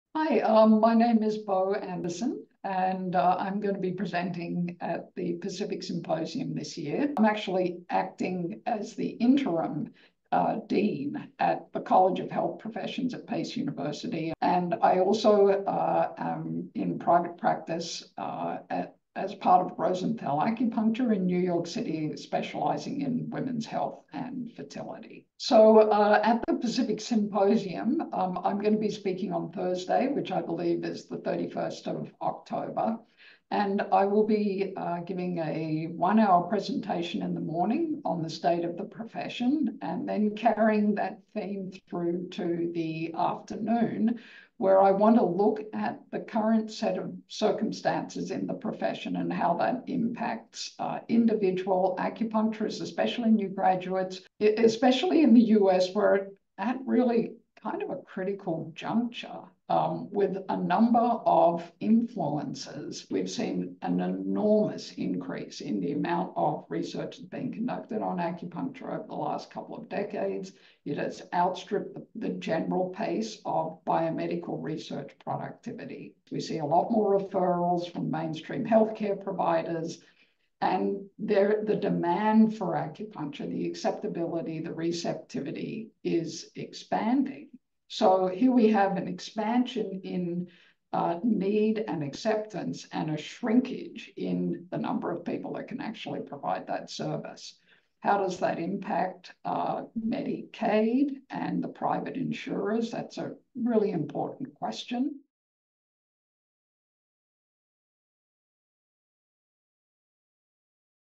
Belinda (Beau) Anderson | State of the Profession | Pacific Symposium 2024 Sneak Peek
Check out this sneak peek of Belinda (Beau) Anderson's presentation at the upcoming Pacific Symposium this fall! Pacific Symposium is the largest acupuncture and Chinese medicine conference in the nation!

This year, Belinda (Beau) Anderson, Ph.D., L.Ac will speak about the current state of the acupuncture profession based on recent developments in the industry.

All courses will be live, with downloadable CEU certificates!